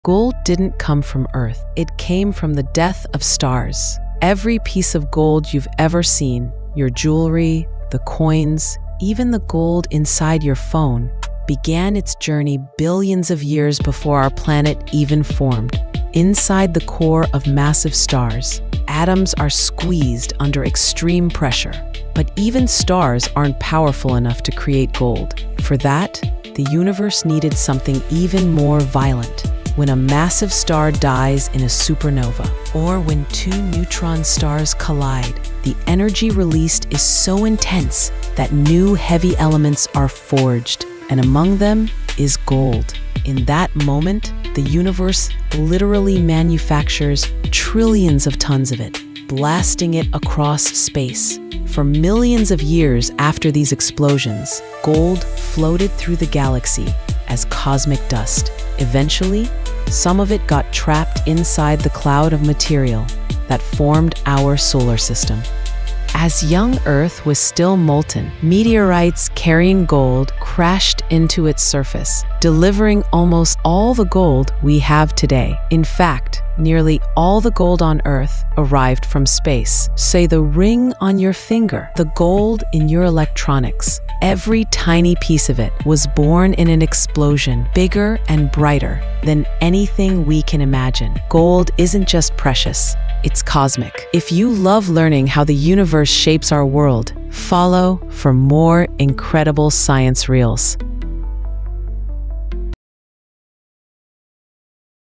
Where Gold Really Comes From?!
Where did all the gold on Earth come from?
The answer doesn’t lie in our planet — it lies in the violent heart of the universe.

Gold is forged in the most extreme events known to science: supernova explosions and neutron star collisions. When these stars die, they release unimaginable energy, creating heavy elements like gold and scattering them across the galaxy.

Billions of years ago, this cosmic gold dust mixed into the cloud that formed our solar system. As meteorites bombarded early Earth, they delivered the gold we find in our crust today.

So the gold we wear, trade, and treasure isn’t just a metal — it's a piece of a dead star, a relic from before Earth even existed.

✨ Gold isn’t earthly — it’s cosmic.

Where facts meet fiction — and every theory has a story.
This is The Nexus Report.